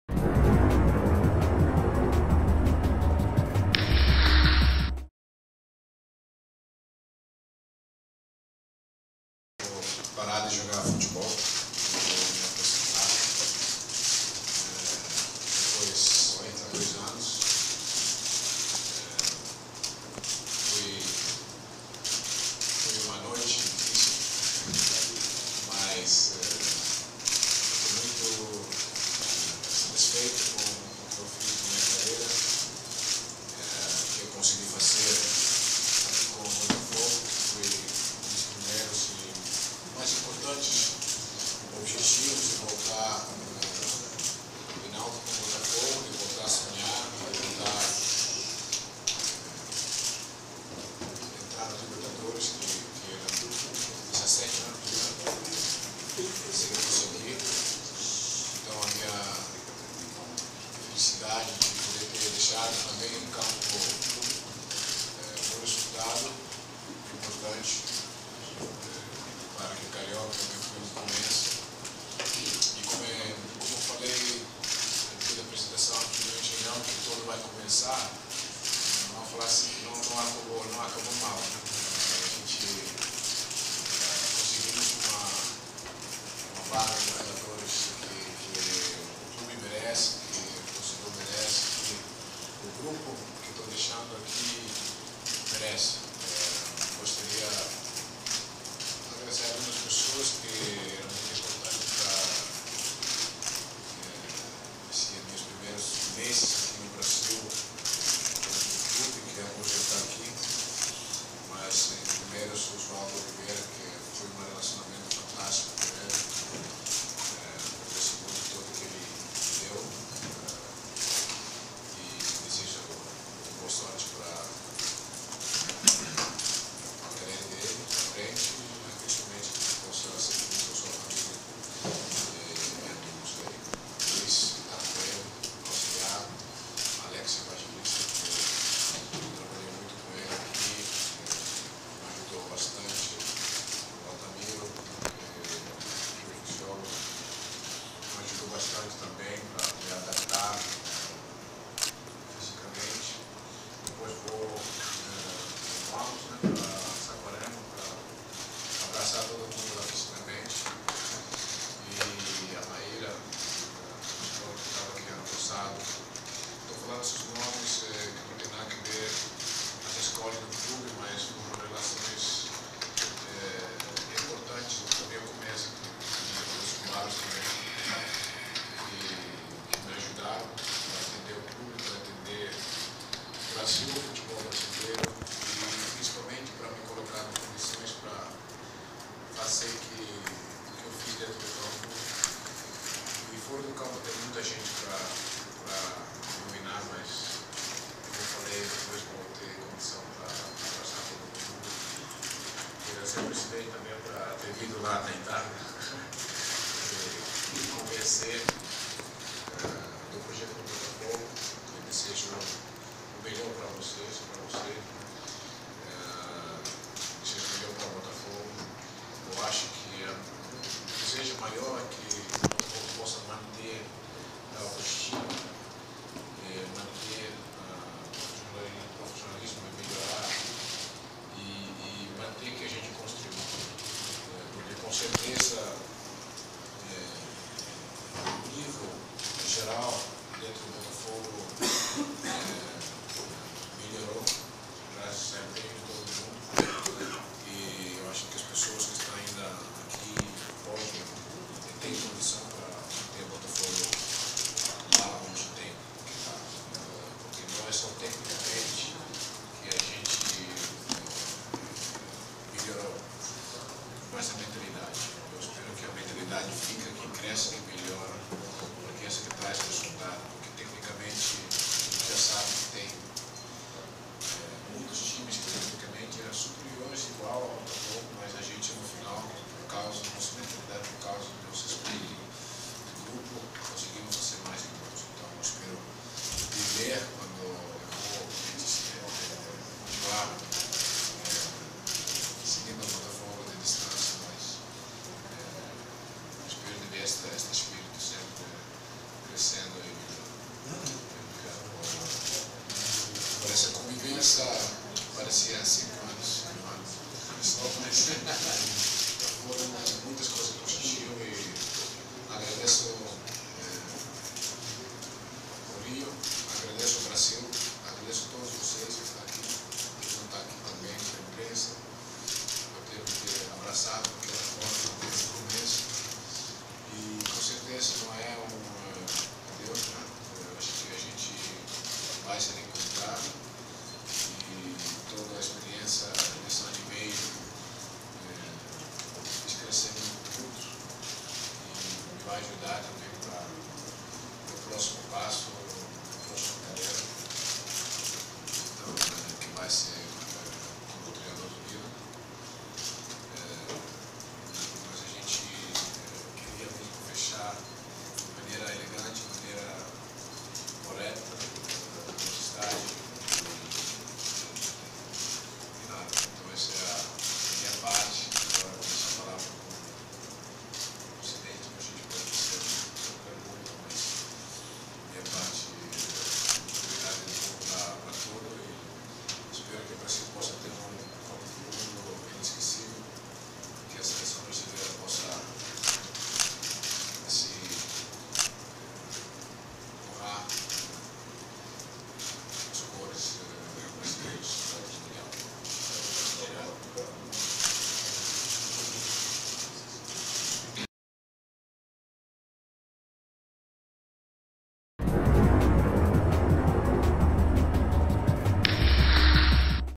Seedorf dá adeus ao Botafogo para ser o novo técnico do Milan
Aos 37 anos, holandês anuncia aposentadoria para substituir Massimiliano Allegri, no clube que defendeu por dez anos.

Crédito: Diego Lopes
Edição: Vinícius Loureiro